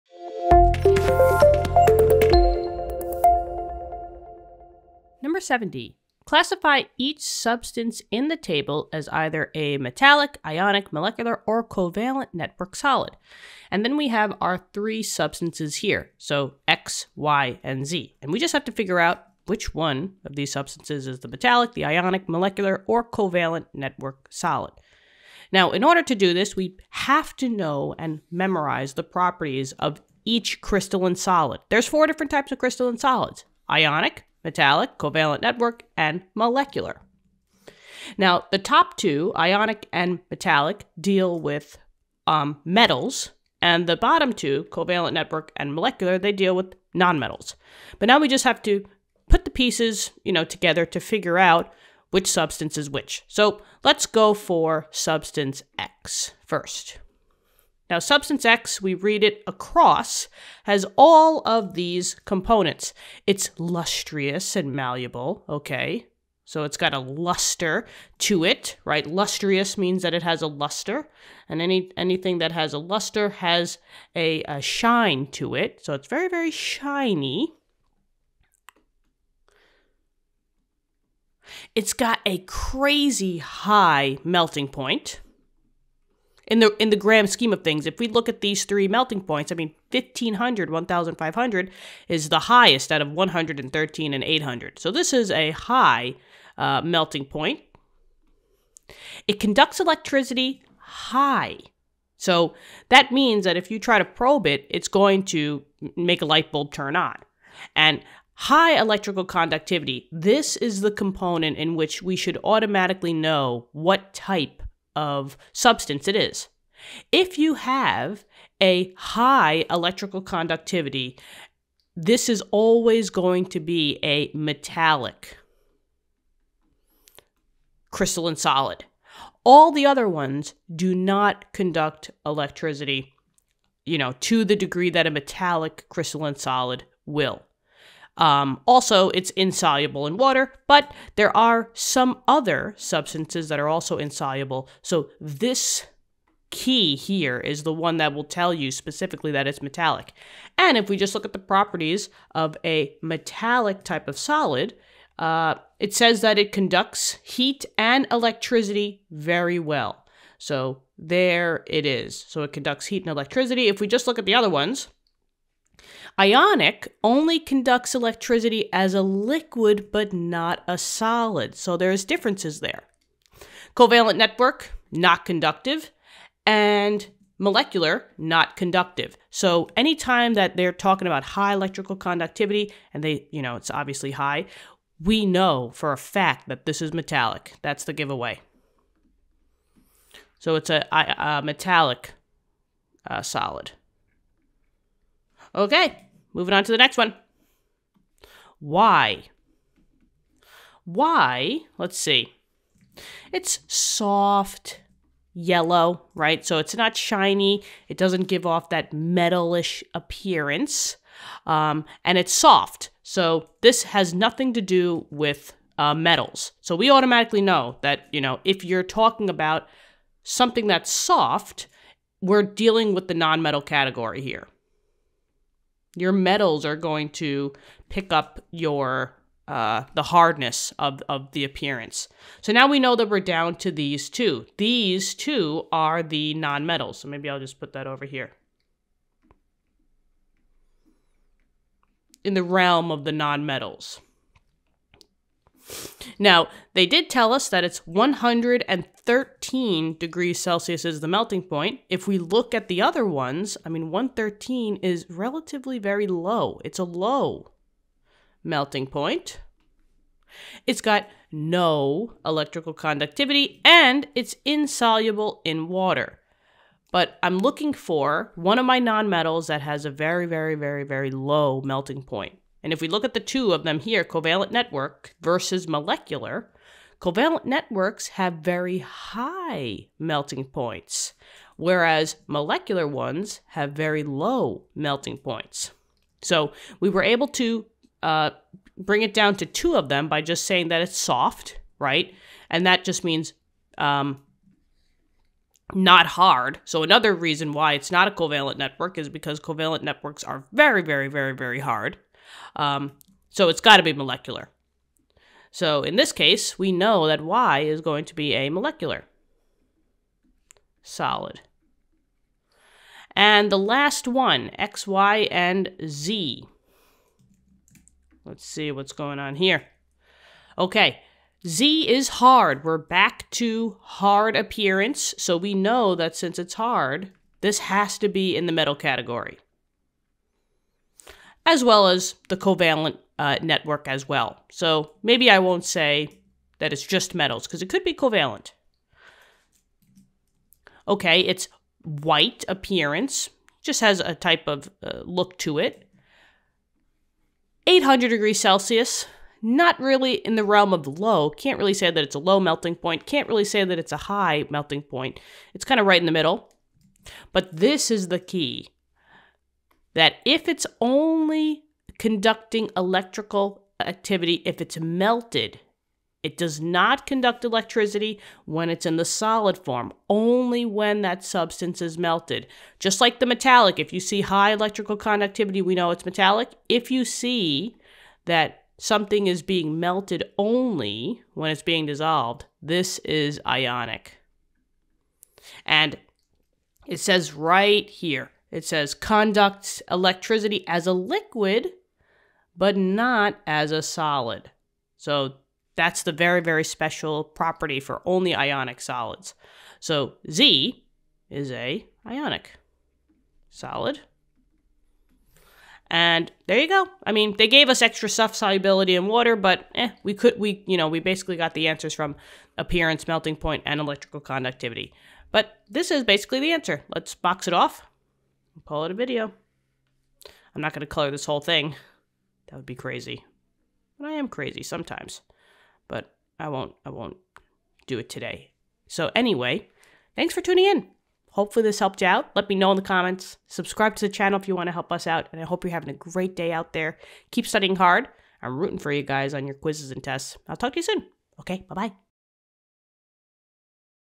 10.70 | Classify each substance in the table as either a metallic, ionic, molecular, or covalent
Classify each substance in the table as either a metallic, ionic, molecular, or covalent network solid: